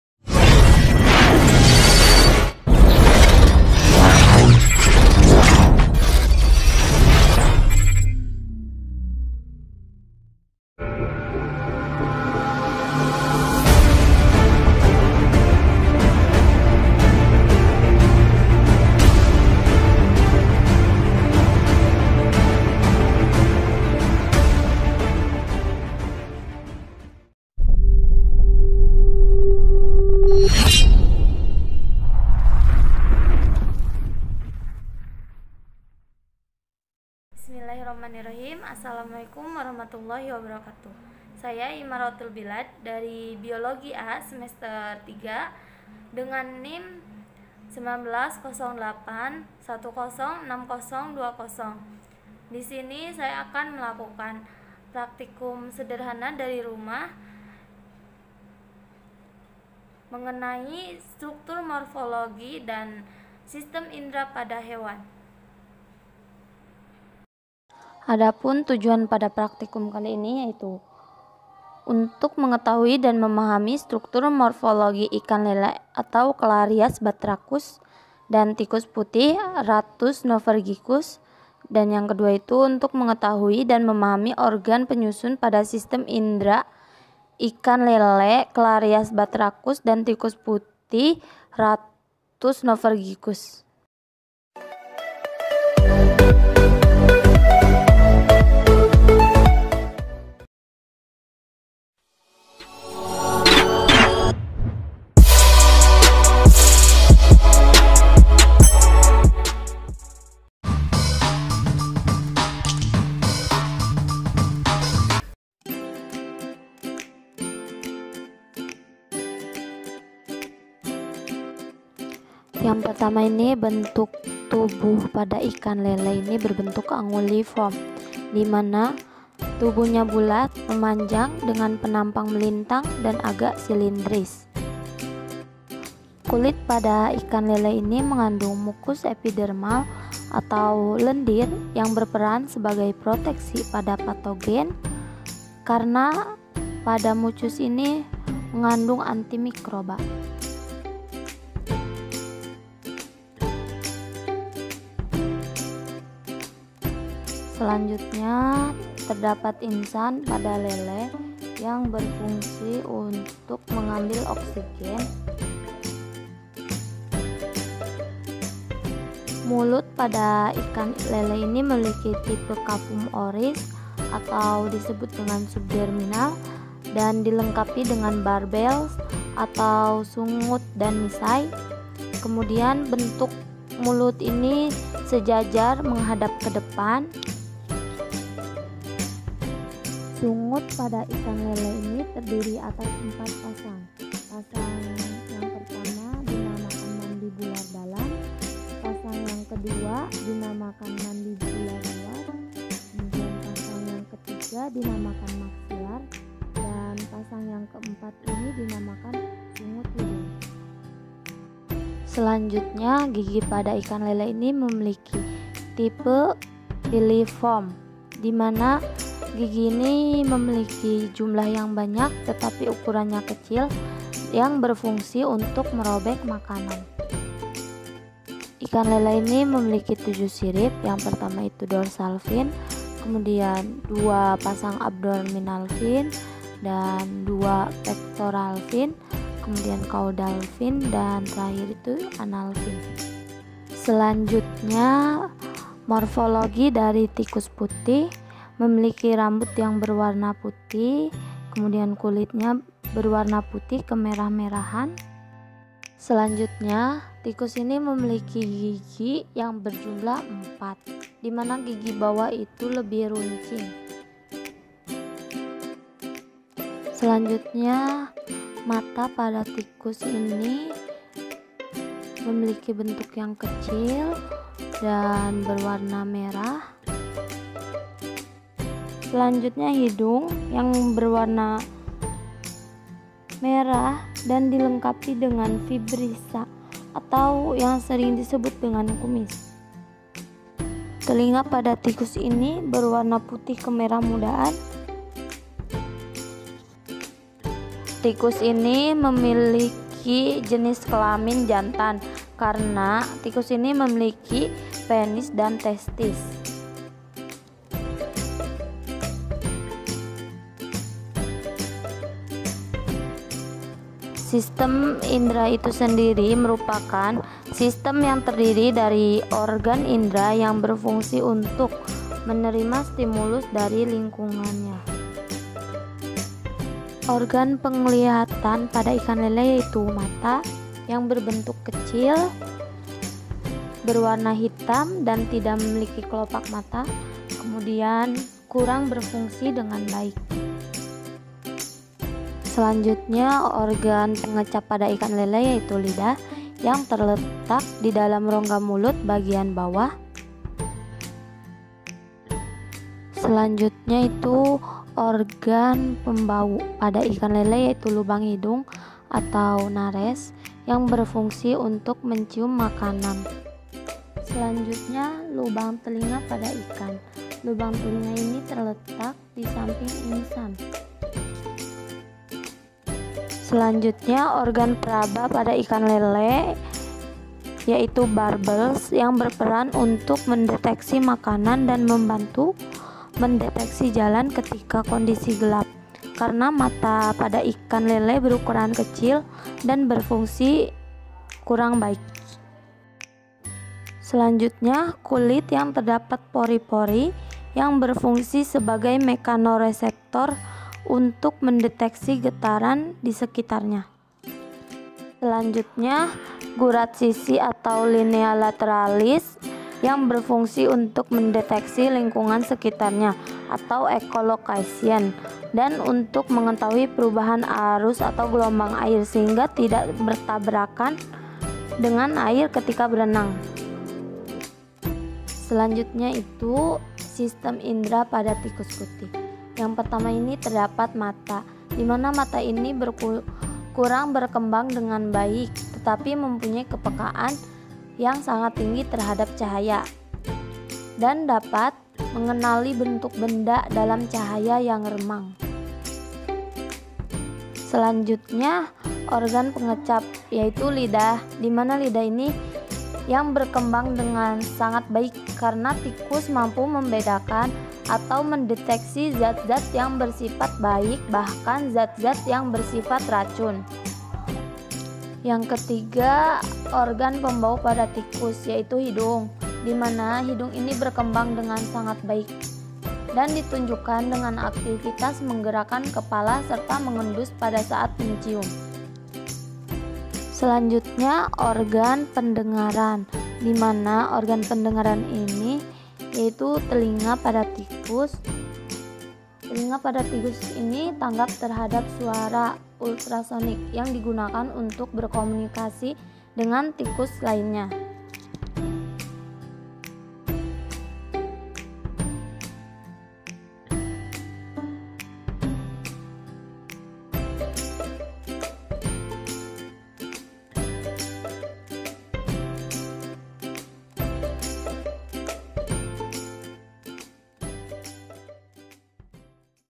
praktikum ke-3 SPH| STRUKTUR MORFOLOGI DAN ORGAN PENYUSUN SISTEM INDRA PADA HEWAN
Vidio ini untuk memenuhi tugas praktikum mata kuliah "Struktur dan Perkembangan Hewan"
*Dosen Pengampu:
1. Shofwatun Nada, M.Pd
2. Muhimatul Umami, M.Si
*Asisten Praktikum:
1. Ajeng Mudaningrat
2. Nindi Fuji Oktoviani
3. Nurul Anwar
4. Uyumatul Umah
*Acara Praktikum Ke-2 : "Sistem Integumen"
* Tujuan Praktikum:
1. Untuk mempelajari dan mengamati struktur histology dan anatomi sistem integumen dan derivatnya
* Praktikan:
Nama: Imarotul Bilad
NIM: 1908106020
Kelas/Semester : Biologi A/3

UNIT LABORATORIUM MIPA INSTITUT AGAMA ISLAM NEGERI (IAIN) SYEKH NURJATI CIREBON